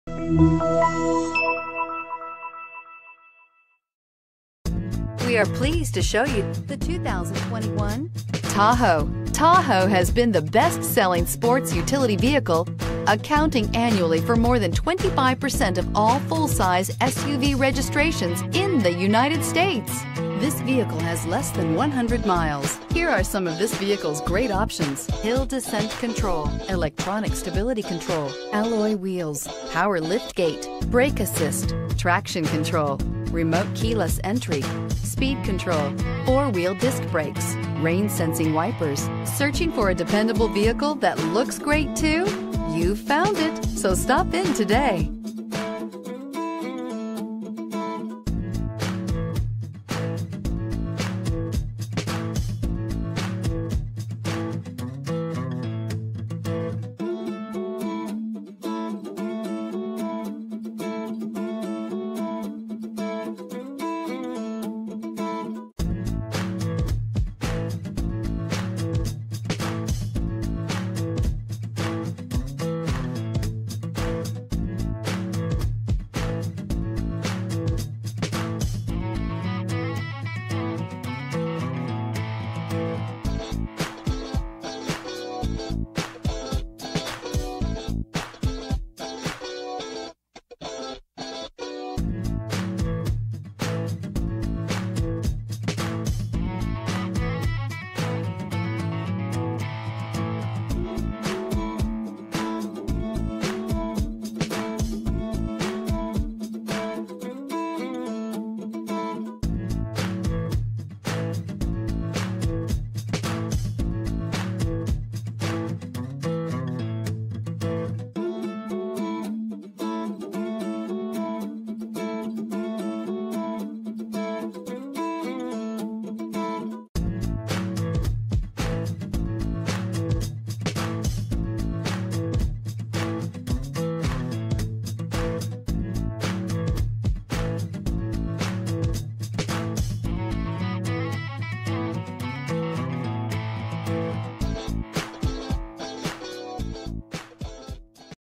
2021 Chevrolet Tahoe 2118953
2021 Chevrolet Tahoe LT Z71

Cavender Chevrolet
30700 Interstate 10 W

Steel 2021 Chevrolet Tahoe LT Z71 4WD 10-Speed Automatic with Overdrive EcoTec3 5.3L V8 4WD, Jet Blk Lth-Appointed.  Recent Arrival!